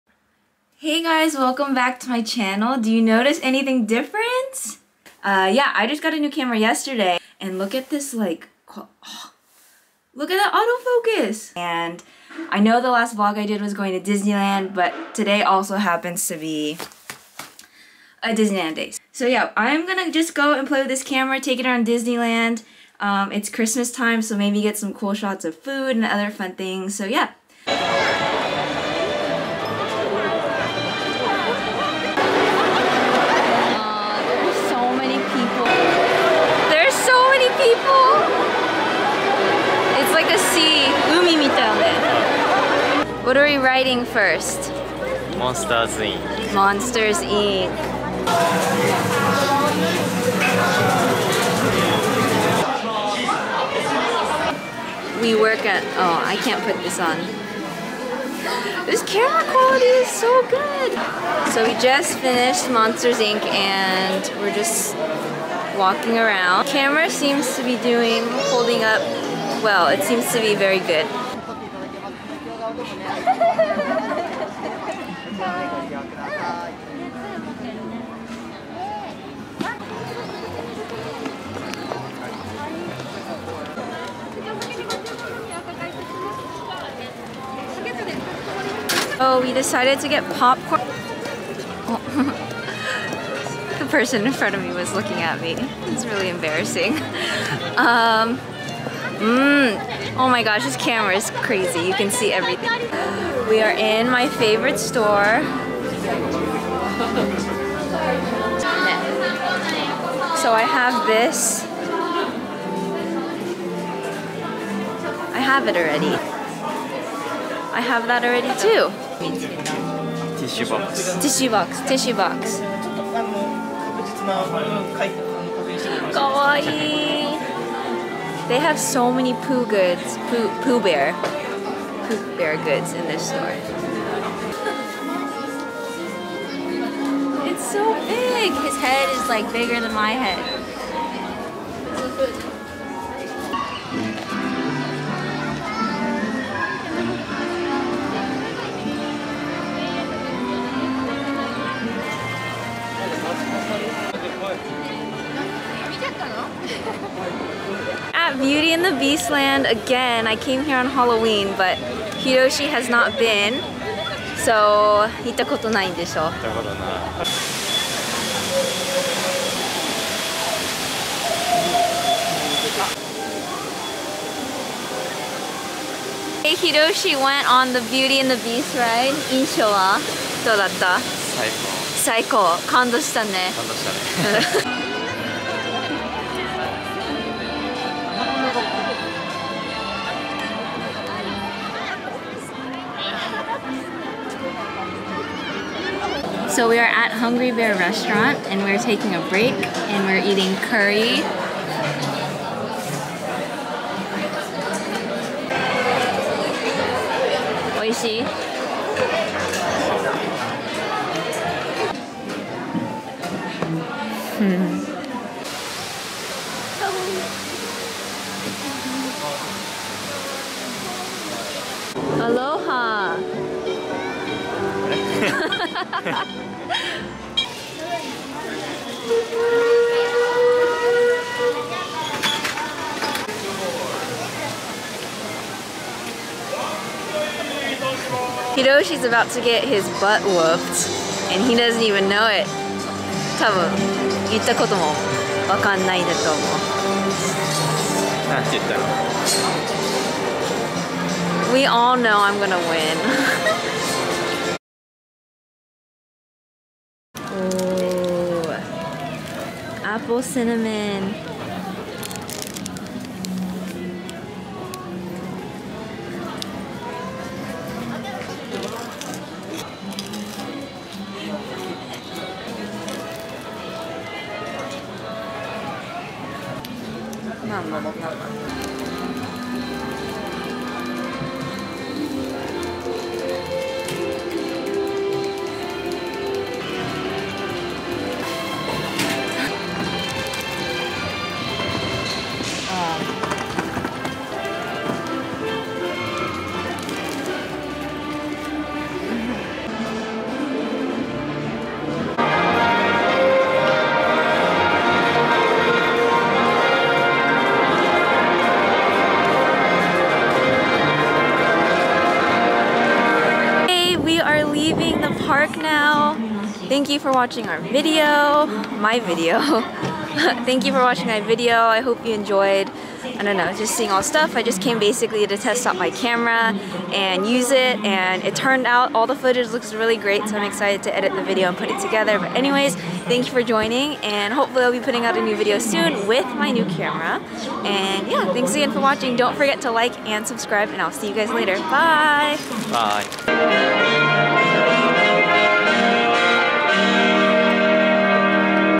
Tokyo Disneyland Using SONY ZV1 BEST VLOG camera??
Tokyo Disneyland Life in Japan VLOG: Shooting with SONY ZV1.
This was my very first day using it so I thought I would take it to Tokyo Disney with me to practice shooting video! Way better than my iphone I was using before... It's a great vlog camera. Still getting used to some settings. Any camera tips would be welcome. Still learning the best way to use it :) Hope to make more life in Japan videos with this camera. Thanks guys enjoy!

Excuse the overdone smooth skin beauty setting...did not realize this from the viewfinder.